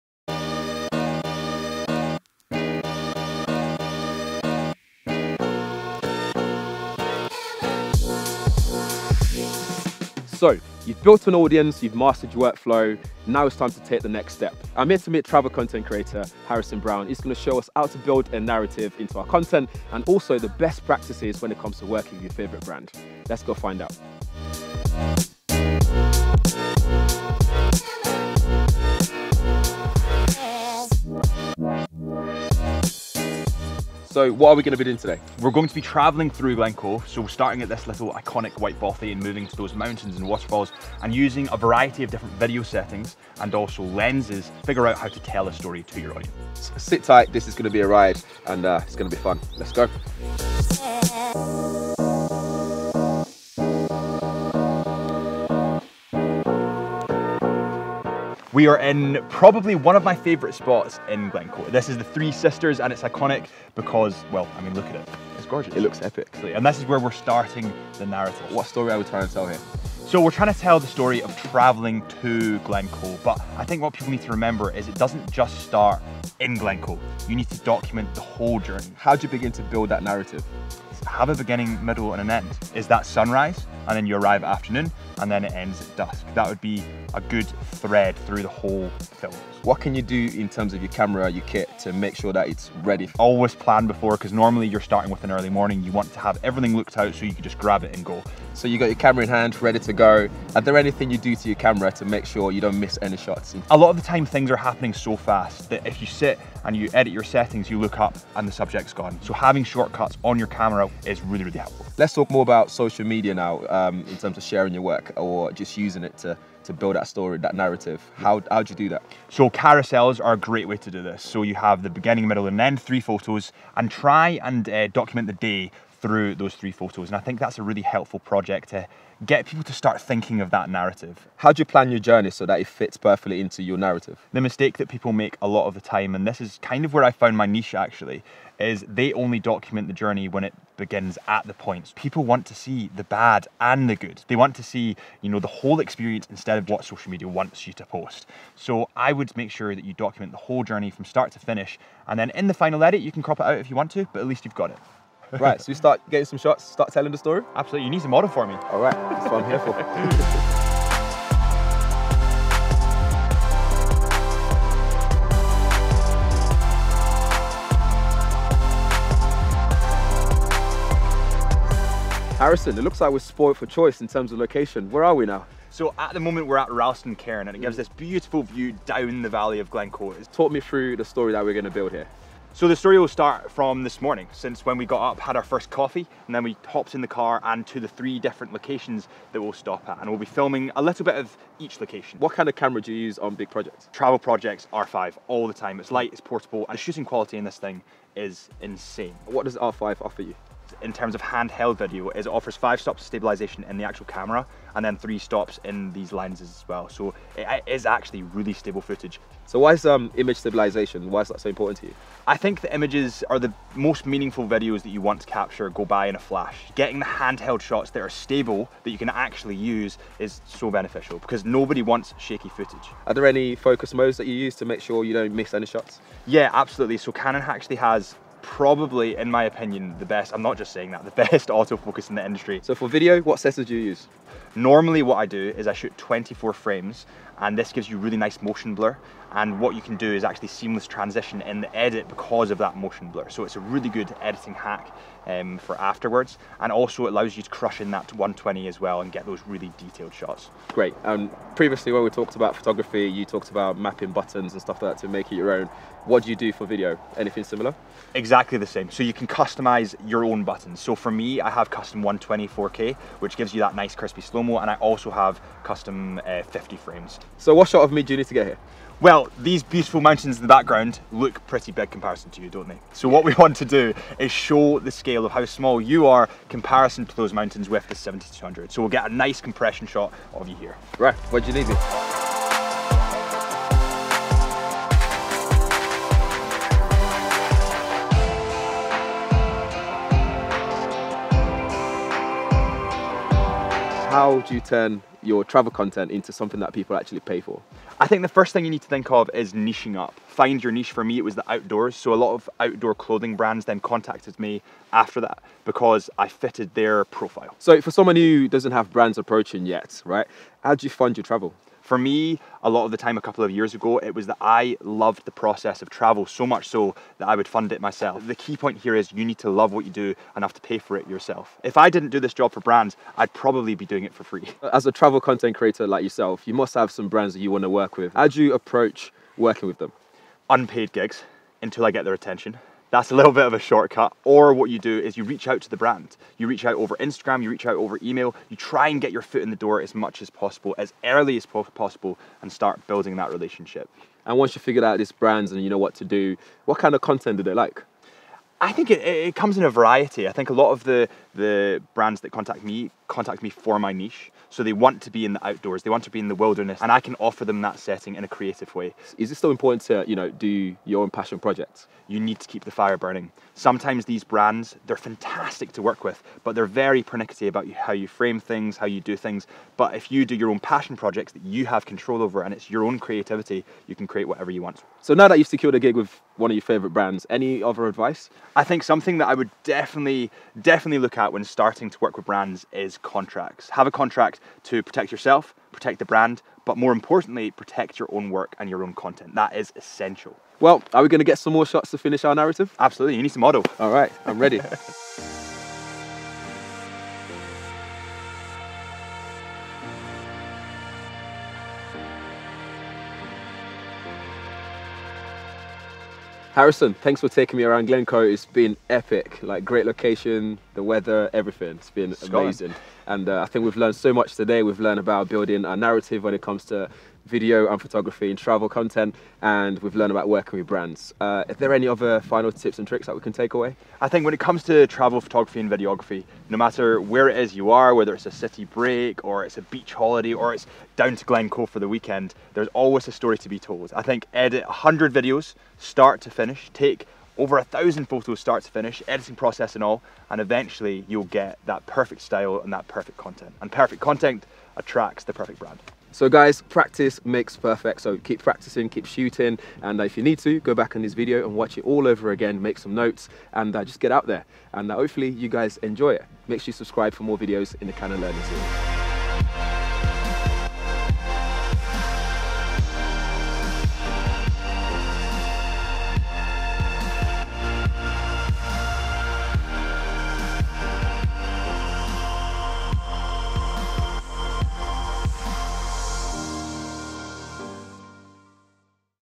Canon Learning Series - Travel Photography – Expert: Telling a story
Travel photography has always been popular, but after the pandemic, many found a new appreciation for travelling, from beginners capturing their memories to experts landing brand collaborations.

To help you progress with your travel content, our host, Tomi Adebayo, who you might know as @GadgetsBoy  asked @HarrisonBrownsLife  to share his tips. Harrison is a photographer and videographer based in Scotland, specialising in travel.

00:27 Chapter 1: Introduction
In the final episode, Harrison will walk you through the steps of how to build a narrative and work together with your favourite brands.

00:54 Chapter 2: Travel narrative
Harrison gives tips on building a narrative and capturing the shots for the story you had in mind.
But he doesn’t stop there, and helps you with sharing your story on social media, including his favourite tried and tested formats on the platforms.

02:48 Chapter 3: Video content and settings
As you’re now an expert building your own brand, your kit will need to reflect that. Harrison’s choice of camera is the EOS R5 for the best quality stills and video.
Harrison will also walk you through his favourite video settings to never miss a moment worth capturing.

04:51 Chapter 4: Working with brands
Finding your niche is key in establishing your personal brand, that’ll then land you your dream collaborations with the brands you’ve always wanted to work with.
But the dream has practical elements to it, Harrison shares a few tips on this as well.

06:43 Chapter 5: Summary
Let’s look at what we’ve learned!